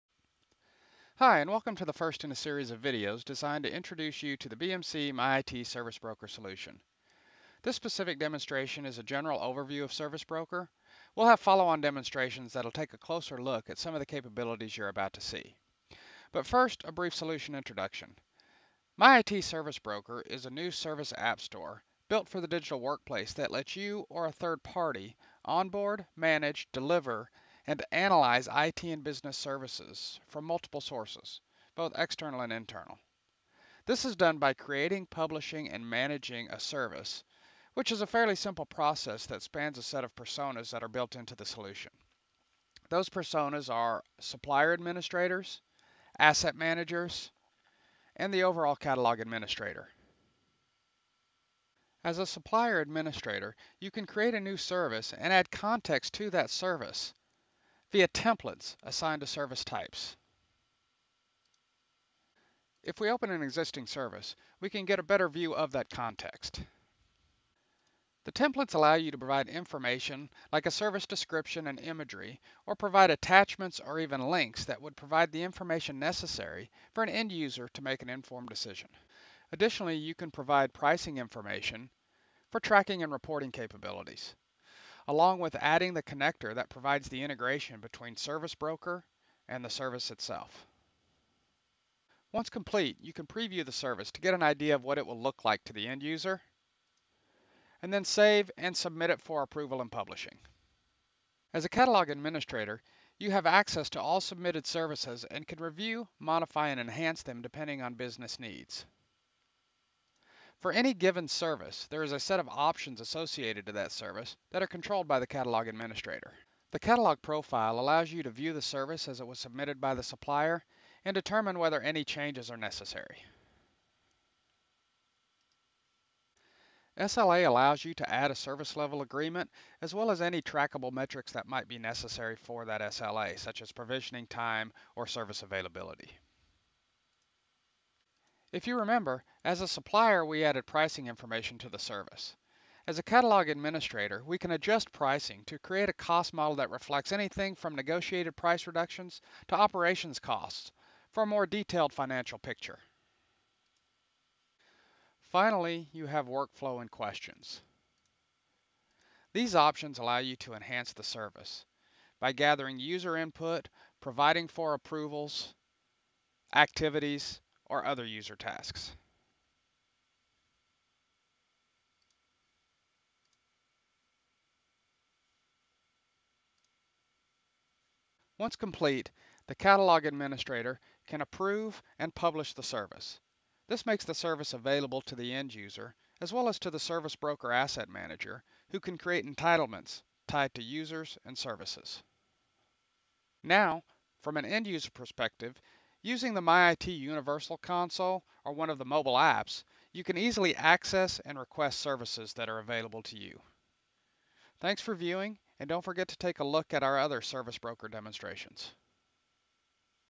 MyIT Service Broker Demo: Product Overview & Highlights
See how the new service app store solution from BMC, MyIT Service Broker, eliminates catalog sprawl, reins in shadow IT and boosts worker productivity.